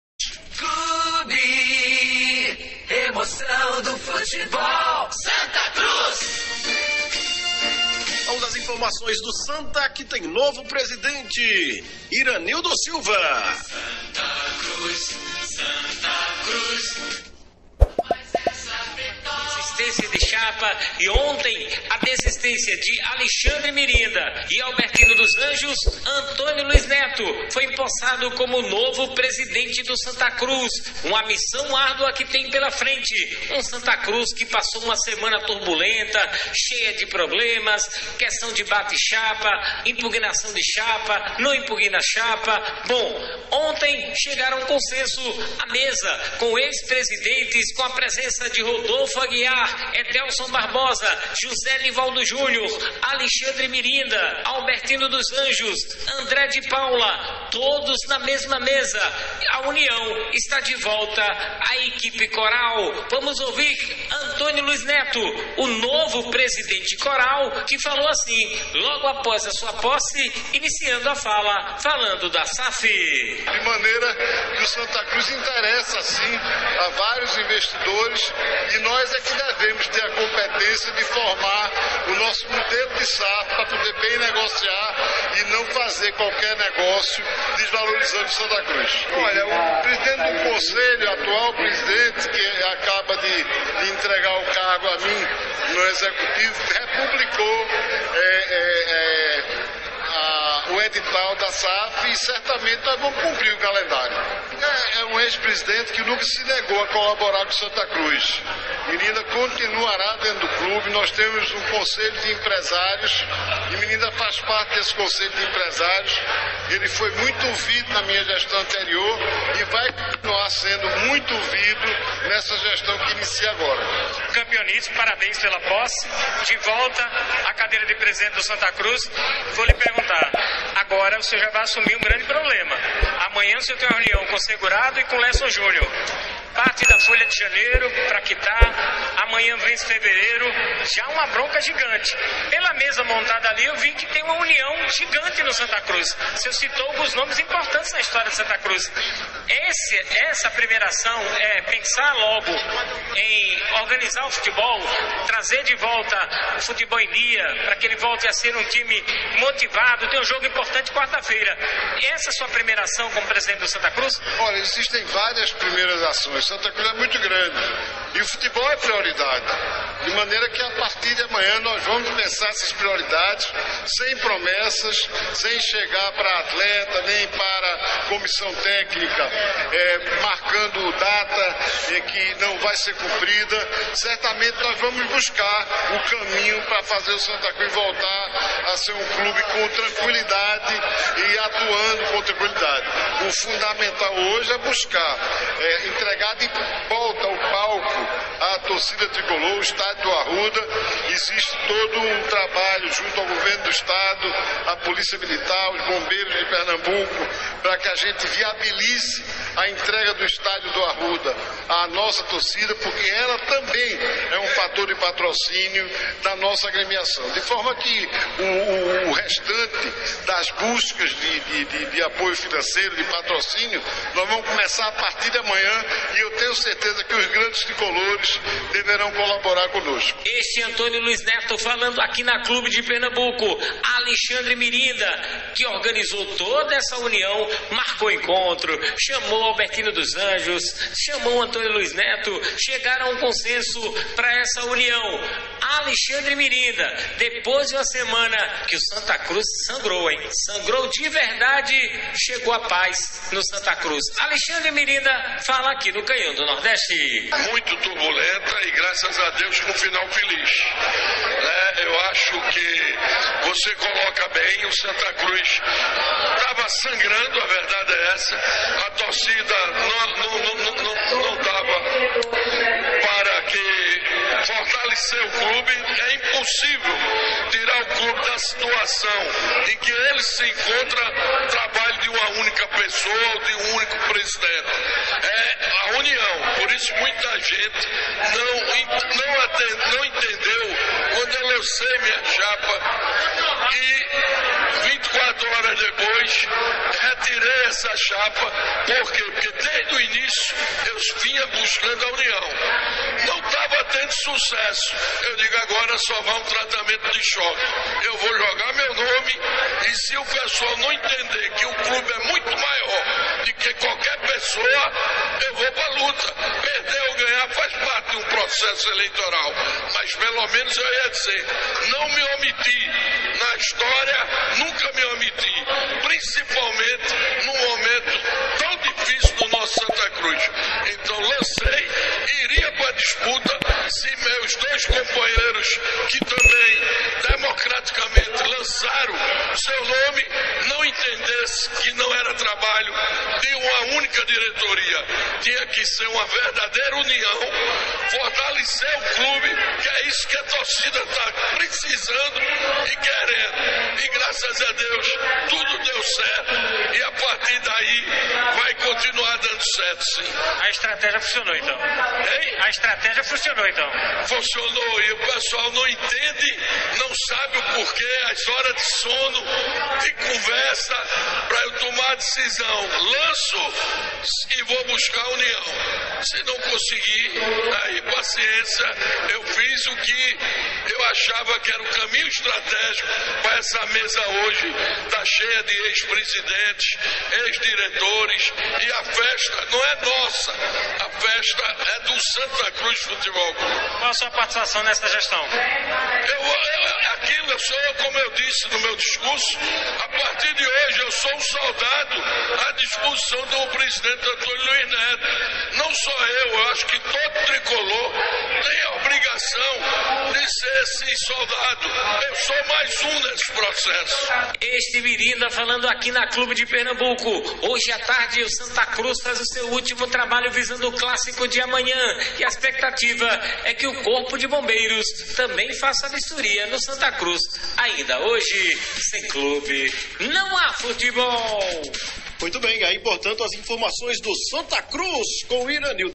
⚠️Antônio Luiz Neto novo Presidente do Santa Cruz ⚠️⚫⚪🔴
🎙️ Notícias do Santa Cruz  ⚫⚪🔴

Repórter 🎙️/ Iranildo Silva

Gravação 🎙️/ GelsonRodrigueSilva

📻 Rádio Clube Recife AM720MHz